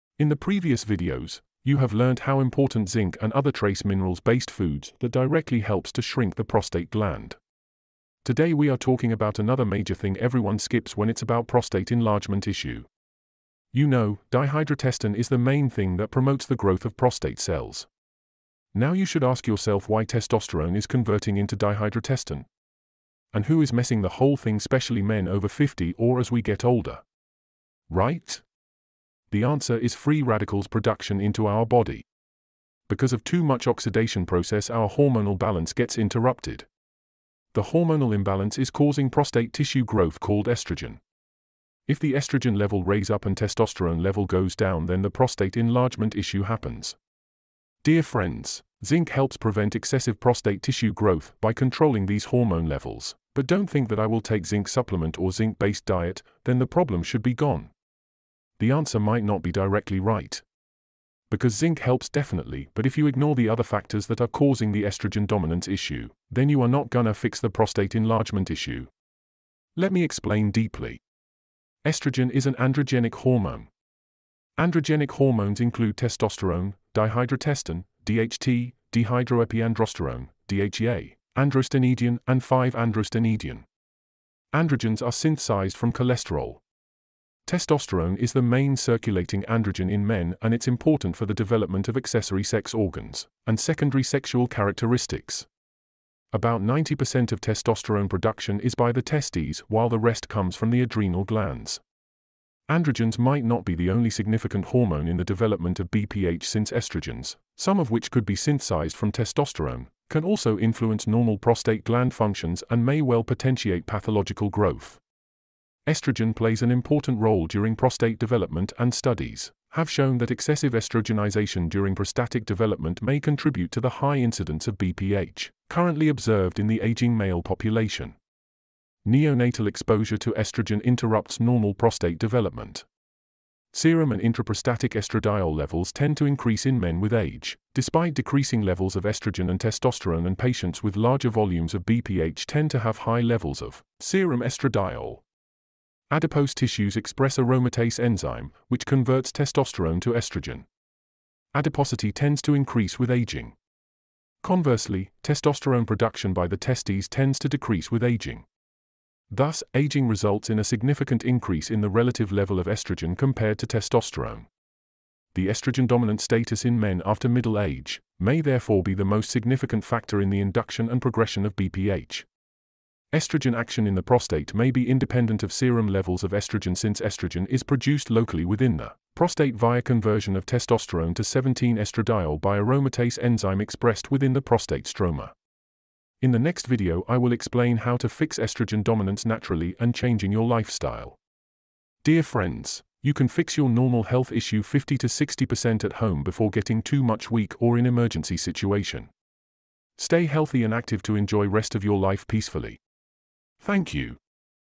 #1 UNTOLD Thing That CAUSES Prostate Tissue Enlargement or BPH
in the previous videos, you have learnt how important zinc and other trace minerals based foods that directly helps to shrink the prostate gland.
today we are talking about another major thing everyone skips when it's about prostate enlargement issue.

you know, dihydrotestosterone is the main thing that promotes the growth of prostate cells. now you should ask yourself why testosterone is converting into dihydrotestosterone? and who is messing the whole thing specially men over 50 or as we get older, right!

the answer is Free Radicals production into our body. because of too much oxidation process our hormonal balance gets interrupted. The Hormonal imbalance is causing prostate tissue growth called estrogen. If the Estrogen level raise up and testosterone level goes down then the prostate enlargement issue happens. Dear friends, Zinc helps prevent excessive prostate tissue growth by controlling these hormone levels but don't think that I will take zinc supplement or zinc based diet, then the problem should be gone?
the answer might not be directly right. Because Zinc helps definitely but if you ignore the other factors that are causing the estrogen dominance issue then you are not gonna fix the prostate enlargement issue.